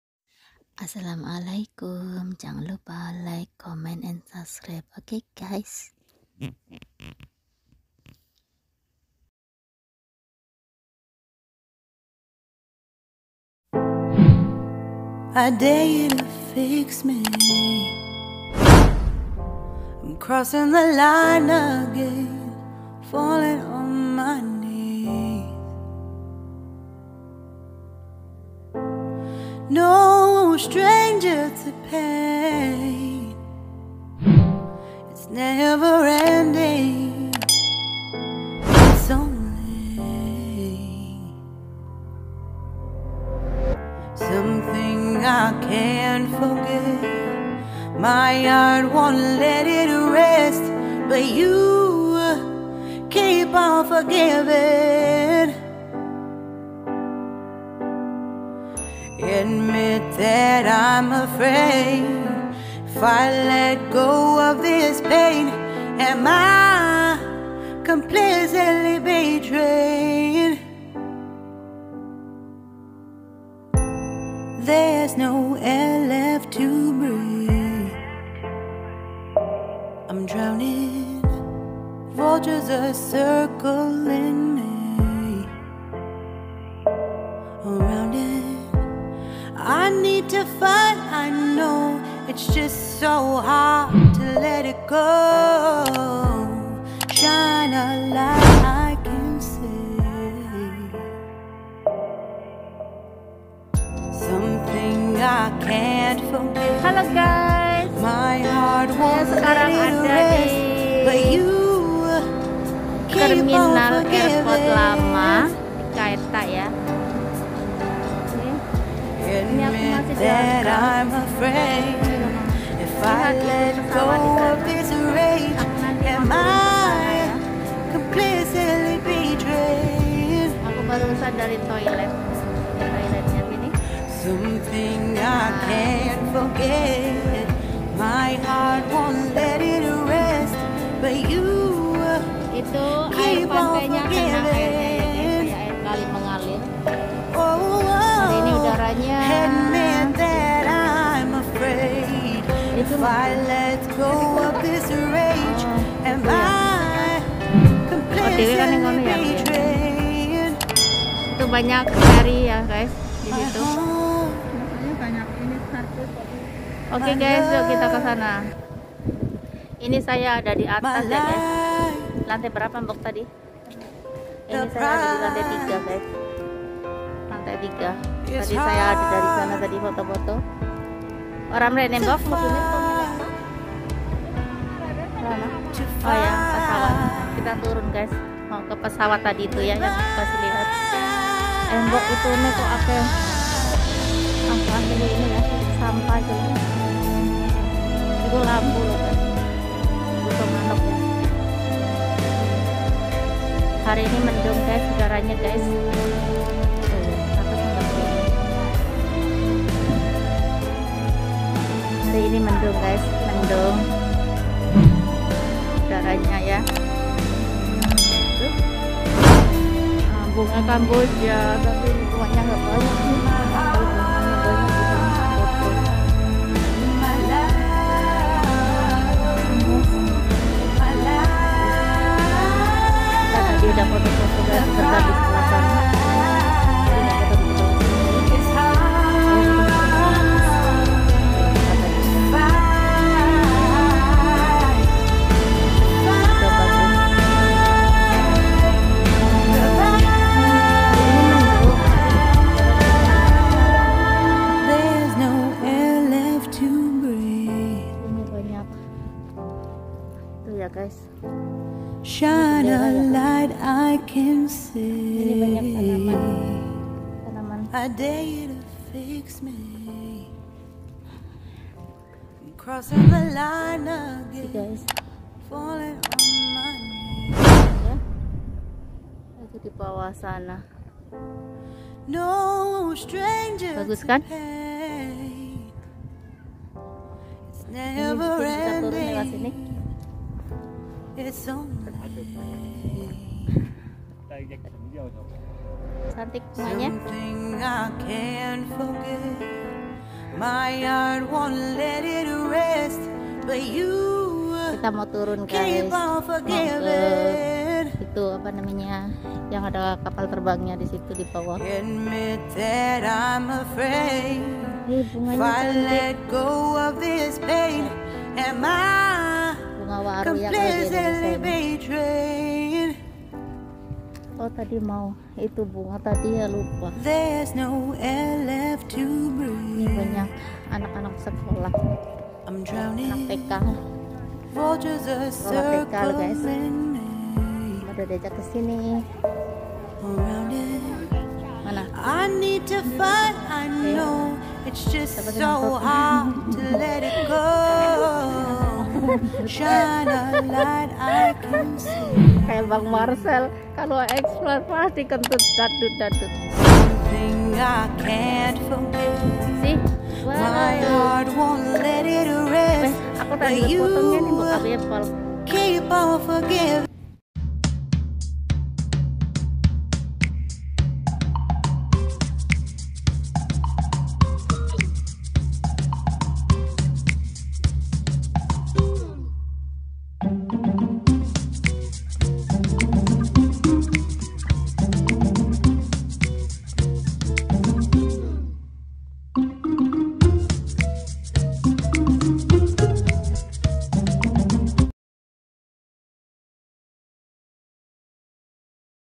We Can Enjoy The Beautiful View From The 3rd Floor Of The Old KAI TAK Airport
Enjoy the beautiful view from the 3rd floor of the old KAI TAK airport
  BEAUTIFUL VIEW we can see the city "in Hong Kong from this place